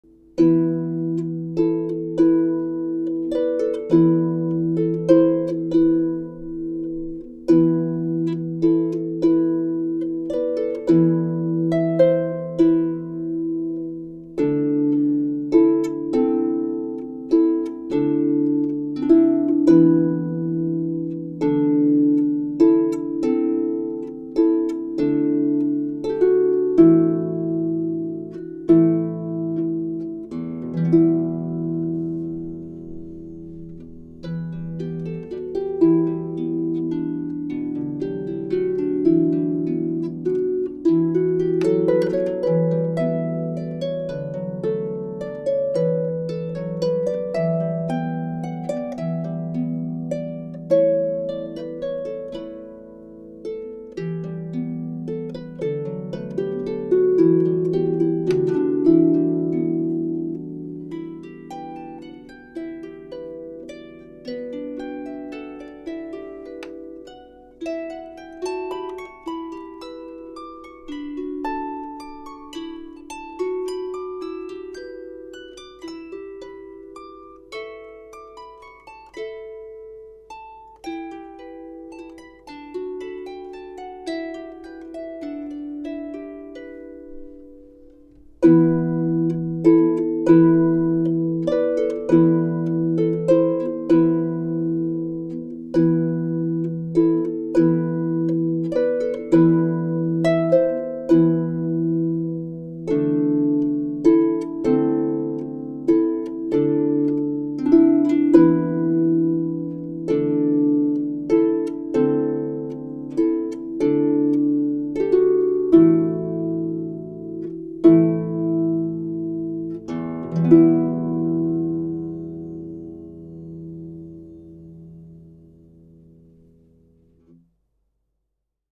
Magical Harp Music for Capricorn Season - Nina Dante
A magical original tune celebrating Capricorn Season in all its chimeric, earthy, noble and magical wintry splendor. The chimeric nature of the Capricorn (or the Seagoat) imbues it with a precious balance that makes it a true earth sign: goat and fish, fire and water, animus and anima, conscious and subconscious, worldly knowledge and intuition.